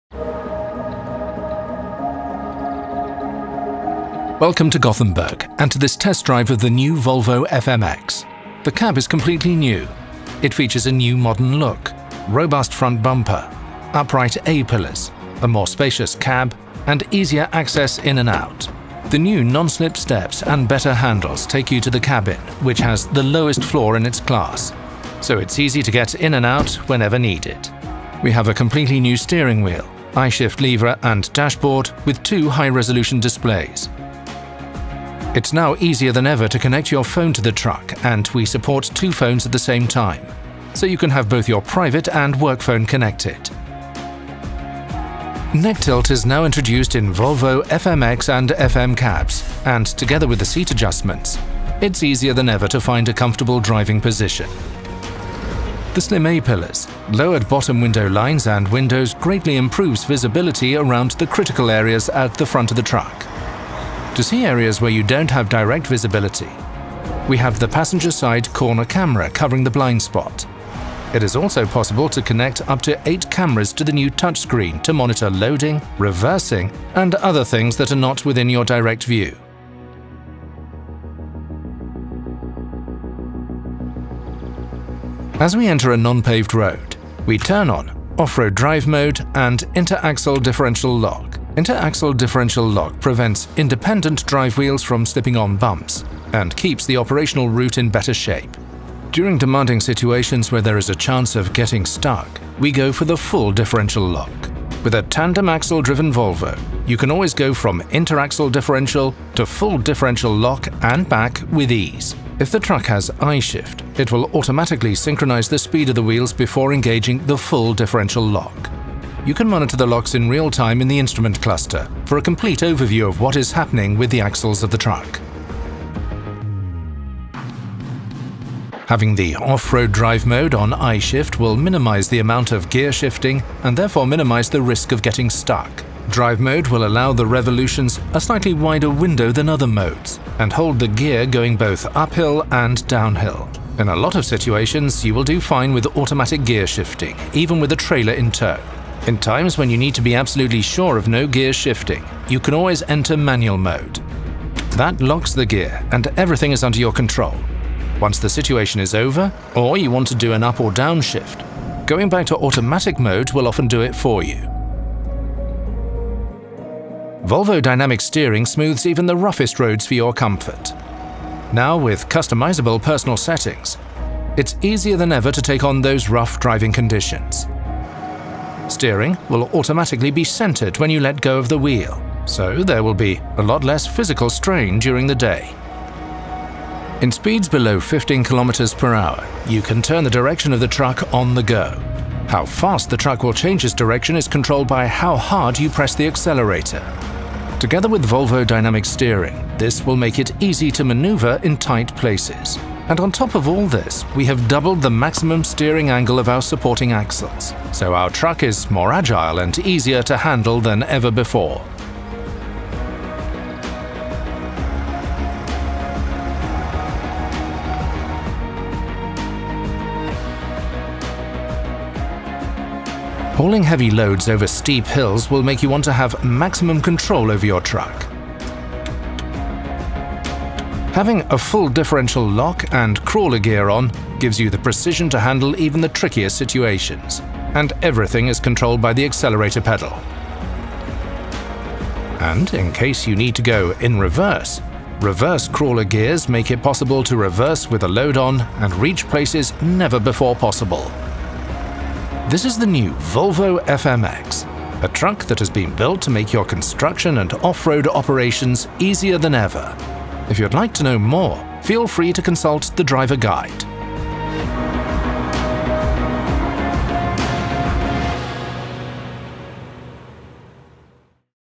Volvo Trucks Test drive of the new Volvo FMX
The new Volvo FMX has more character, more space, and more intelligence built-in – and it’s more efficient than ever.

The cab is completely redesigned and features a new modern look, robust front bumper, and upright A-pillars for a more spacious cab. The door lines at the side windows are lower and give improved visibility.

Inside you will find a completely new steering wheel, I-Shift lever, and dashboard with two high-resolution displays.

Ride along as we take it out for a spin, and show you some of its new features.